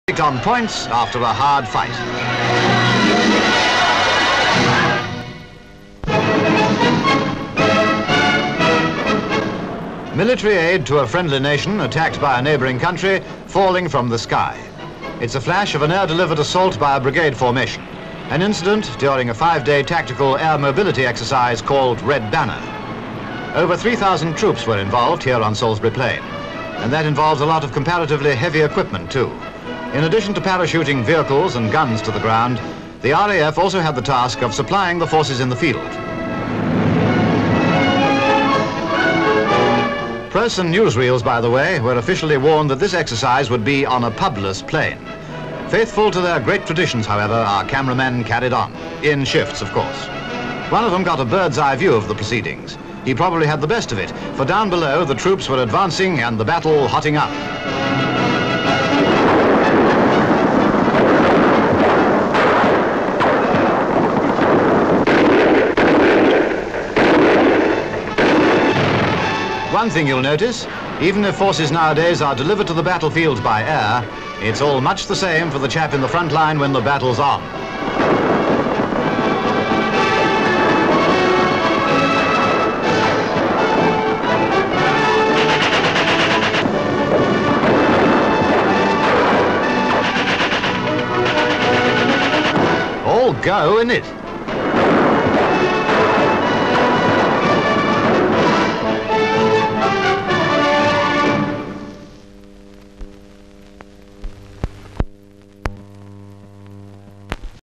EXERCISE "RED BANNER"
(19 Oct 1959) Our pictures show a flash on an air-delivered assault by a brigade formation, an incident during a five-day tactical air mobility exercise, called "Red Banner". Over three thousand troops were involved here on Salisbury Plain. One of Movietone's cameramen got a bird's-eye view of the proceedings.